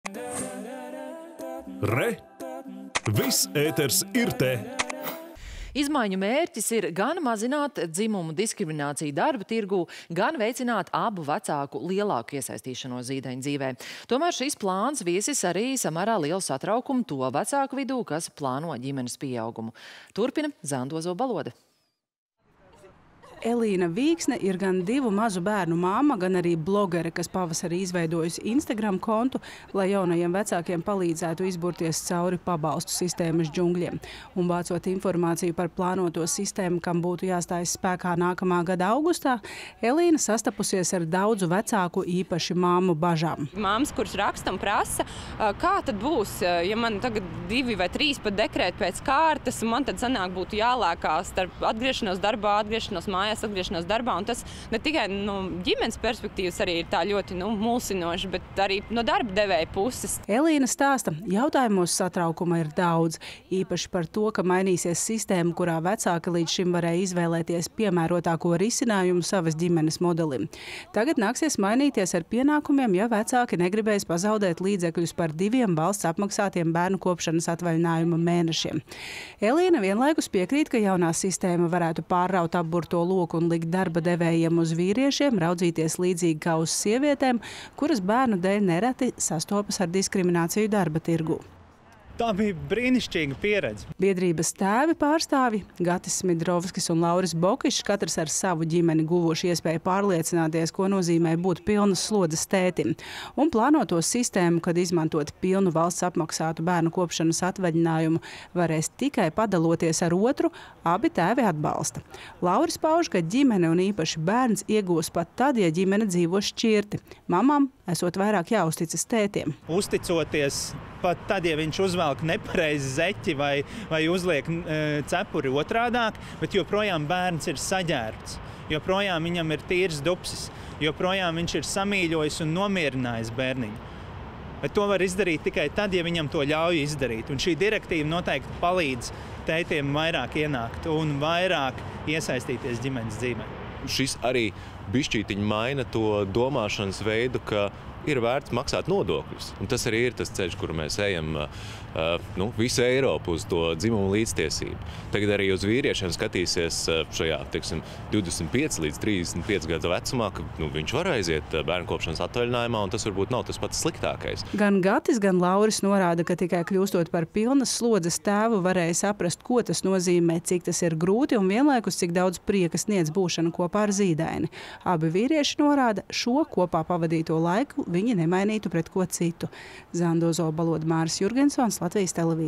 Vecāki bažīgi par izmaiņām vecāku pabalsta sistēmā
Izmaiņu mērķis – gan mazināt dzimumu diskrimināciju darba tirgū, gan veicināt abu vecāku lielāku iesaistīšanos zīdaiņa dzīvē. Tomēr šis plāns viesis arī samērā lielu satraukumu to vecāku vidū, kas plāno ģimenes pieaugumu. Turpina Zanda Ozola-Balode.